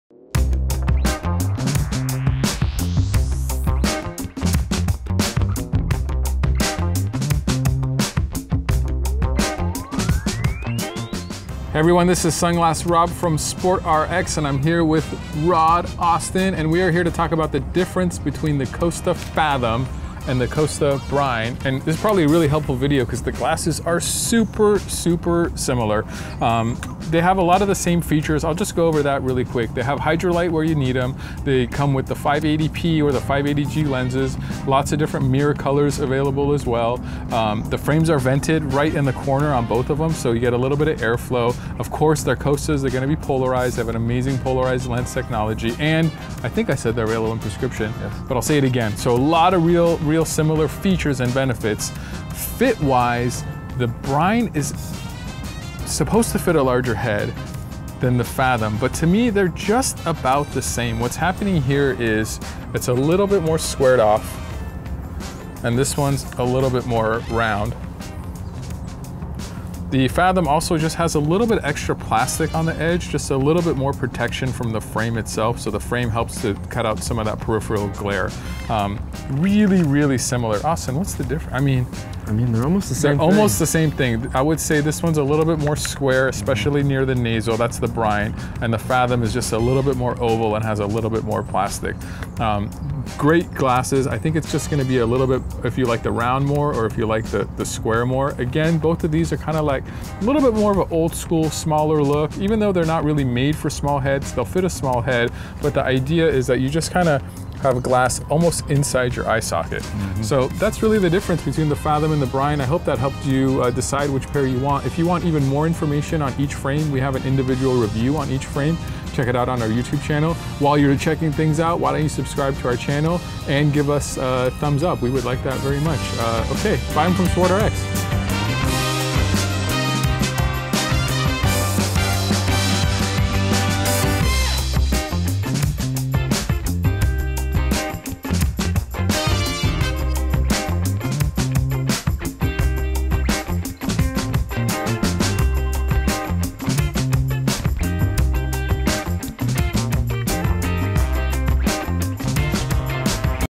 Costa Brine vs. Costa Fathom | SportRx
Sunglass Rob and Platinum Team Member, Austin from SportRx take a closer look at the Costa Fathom and the Costa Brine sunglasses. Costa Fathom sunglasses are a mean looking pair of shades for the die-hard fisherman out there. While the Costa Fathom features a sleek look and optimal protection. Need them in prescription, no problem SportRx has you covered.